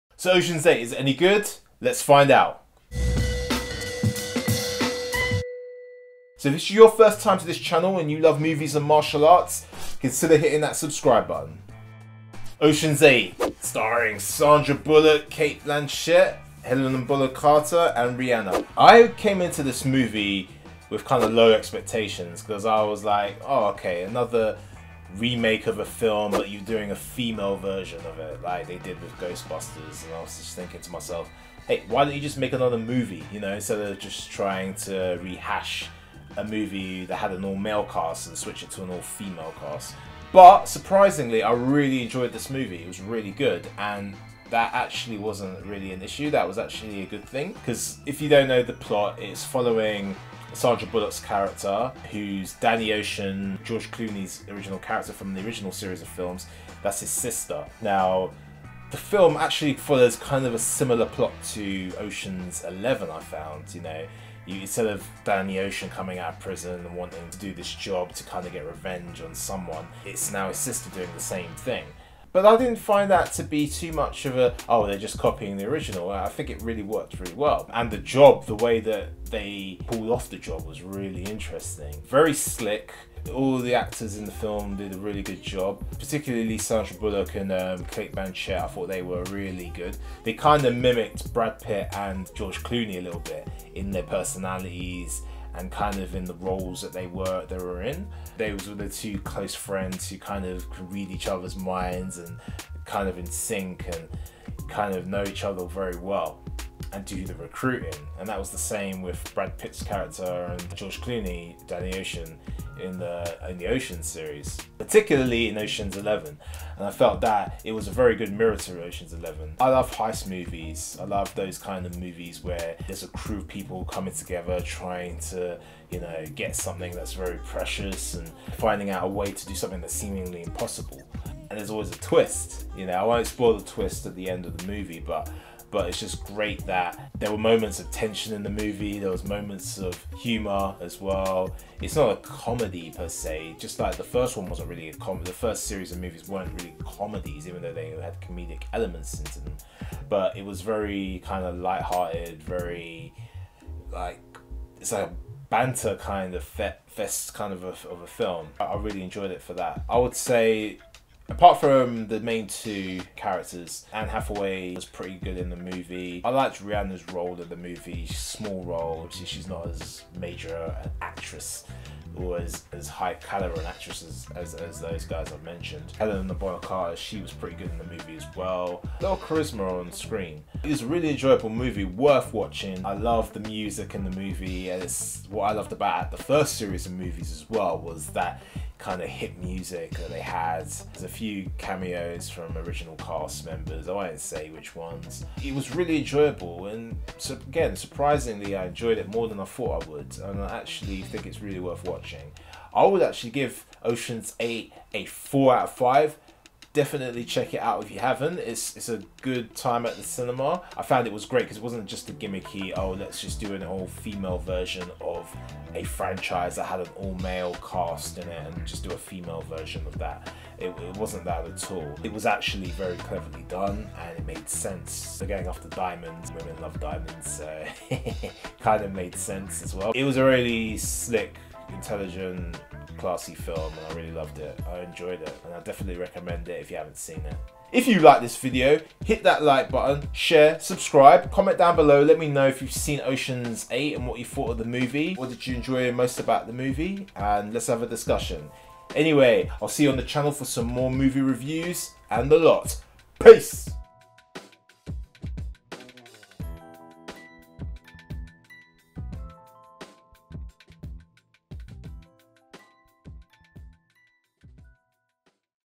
Ocean's 8 - Not Just A Female Version Of Ocean’s 11
Ocean's 8 - Well Worth Watching Tonight

Here is my Ocean's 8 movie review. Starring Sandra Bullock, Cate Blanchett,  Anne Hathaway, Helena Bonna Carter and Rihanna.

Is Ocean's 8 any good? Or just a poor version of the orginal Ocean's series of movies starring George Clooney, Brad Pitt, Matt Damon, Don Cheadle and company? Here are my thoughts on the movie.

If you want to see what it's like to train Muay Thai in Thailand watch this playlist